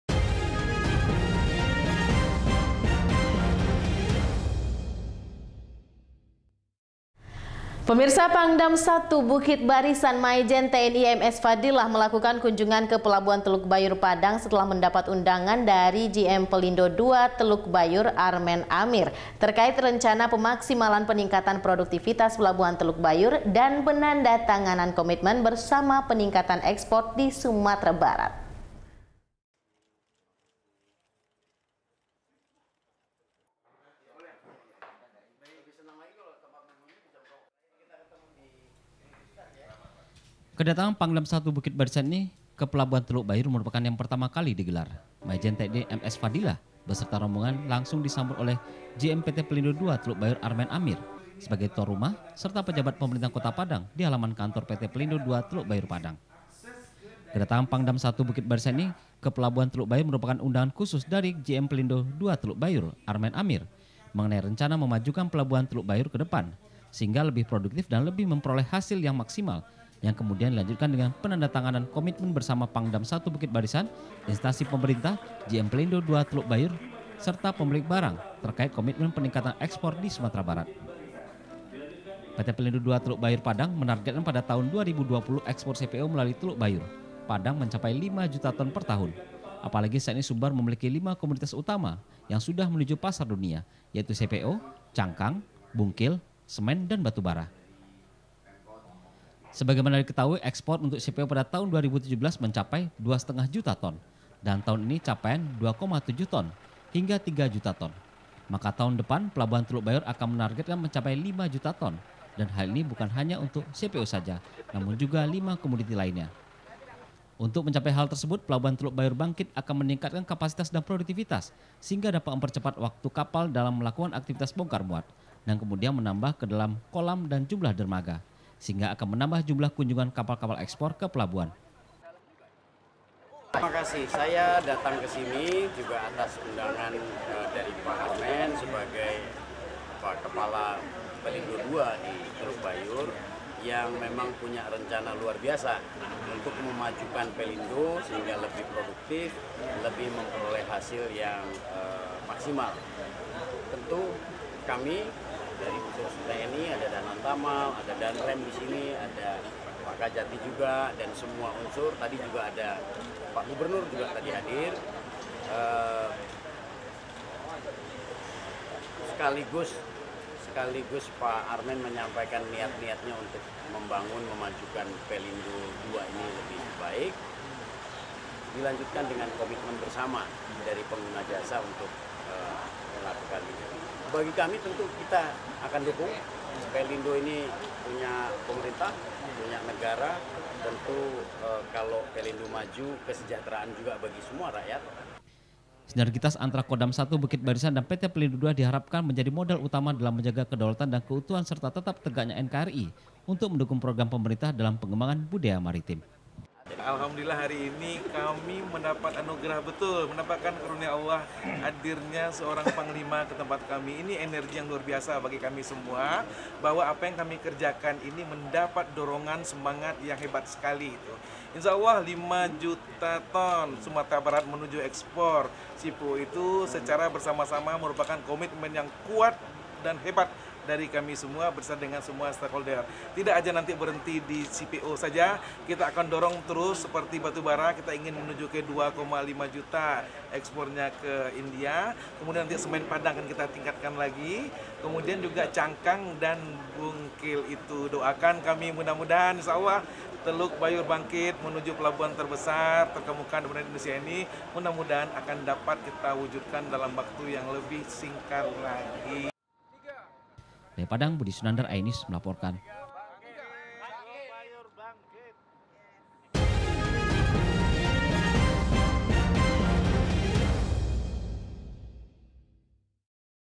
Pangdam I Bukit Barisan Kunjungi PT Pelindo II Teluk Bayur
Pangdam I Bukti Barisan, Mayjen TNI M.S Fadhilah, melakukan kunjungan ke Pelabuhan Teluk Bayur Padang, setelah mendapat undangan dari GM Pelindo II Armen Amir, terkait rencana pemaksimalan peningkatan produktifitas Pelabuhan Teluk Bayur dan penandatanganan komitment bersama peningkatan ekspor di Sumatera Barat.